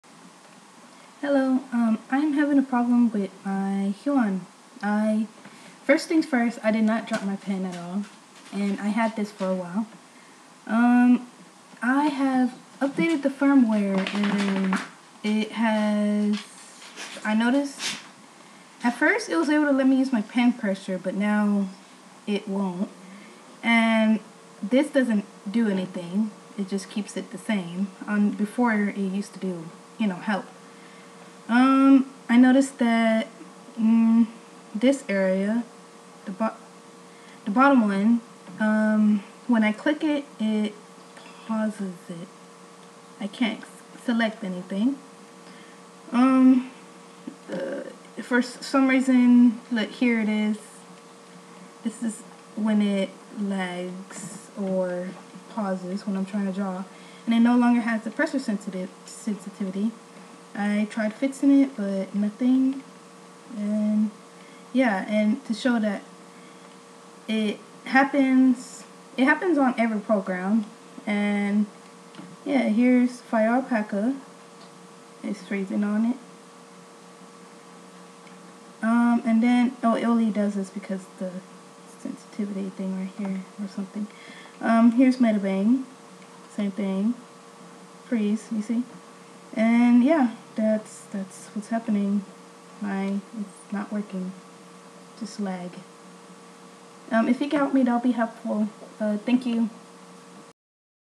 Skipping lagging problem for Huion Kamvas pro 13
Here is another view of the skipping lagging problem. I have further details on what's wrong with it on my other video.